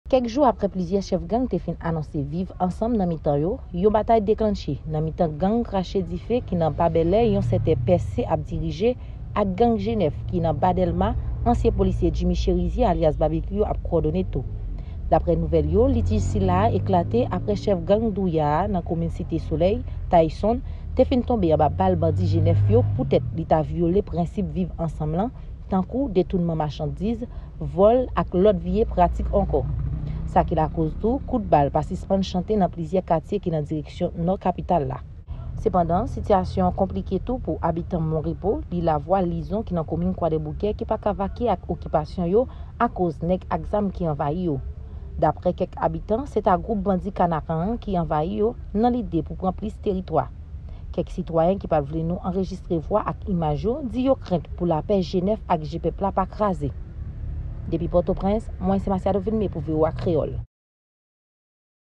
Chef Gang G9 la Dozado ak Gang Je Pep la, Ki Lakoz Anpil Tet Chaje pou Popilasyon an
Chef Gang G9 la nan chire pit ak chef gang Je Pep la, sa ki lakoz popilasyon ki abite nan zon yo kontwole gen anpil tet chaje.